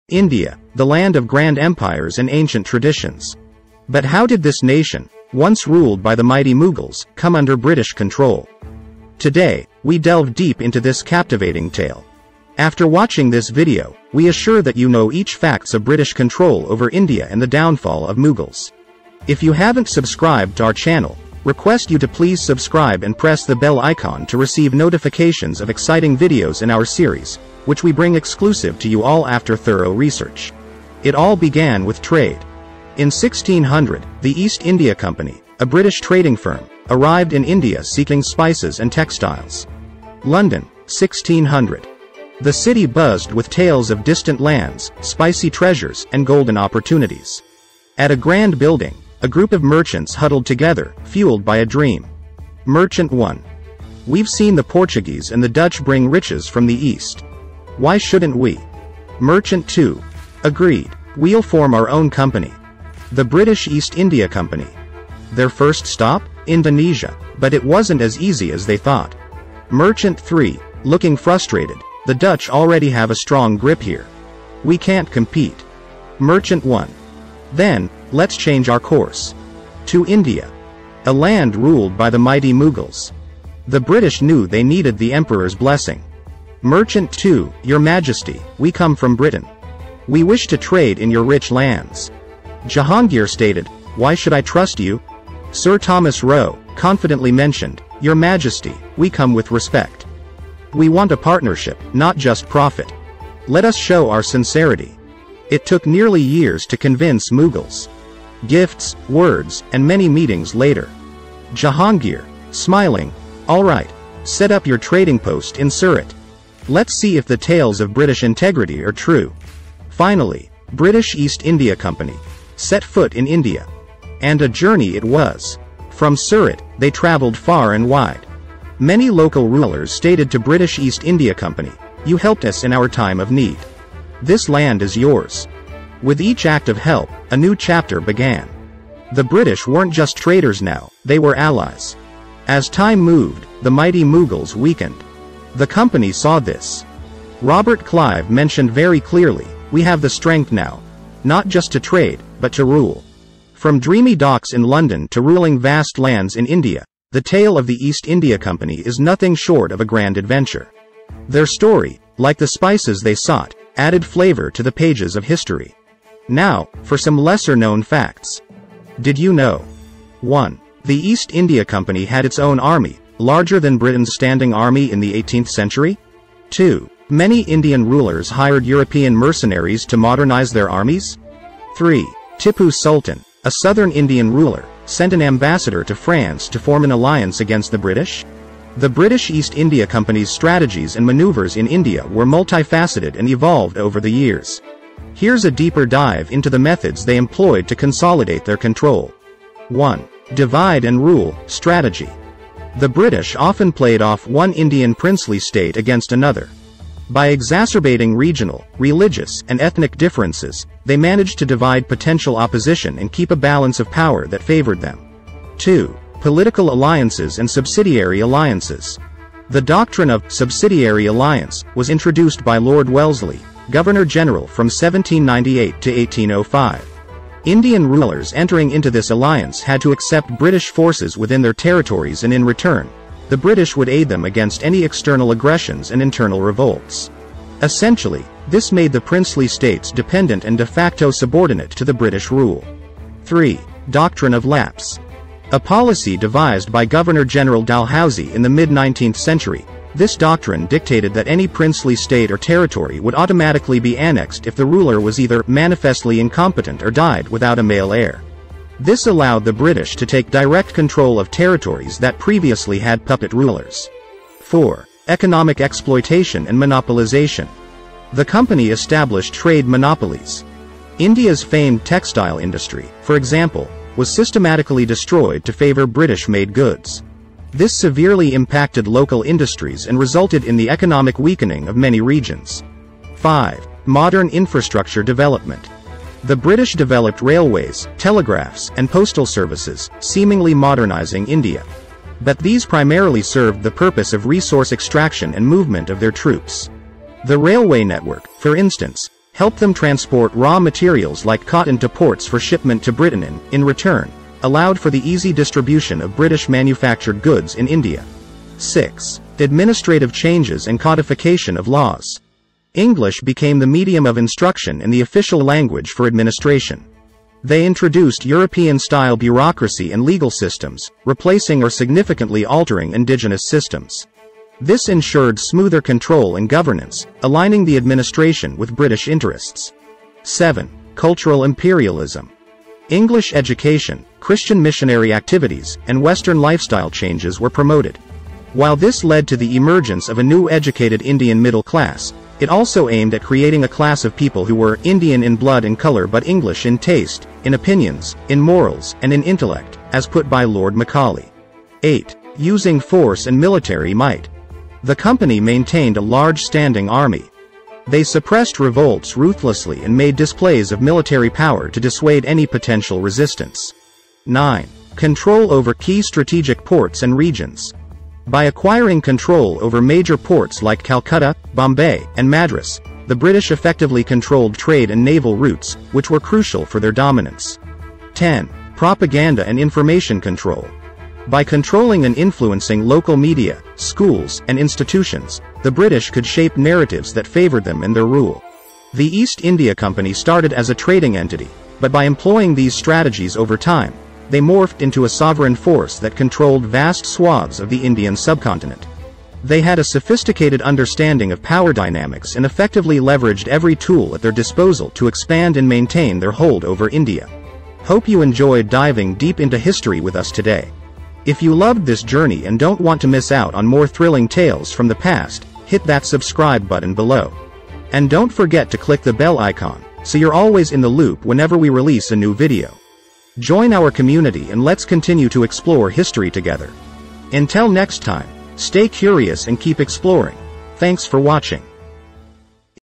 From Trade to Rule: The British East India Company's Rise in India
"Dive into the captivating journey of the East India Company, from their humble beginnings in London to ruling vast lands in India. Discover the strategies, alliances, and key figures that shaped their legacy.

How East India Company Captured India
independence day speech 2023
independence day speech in english 2023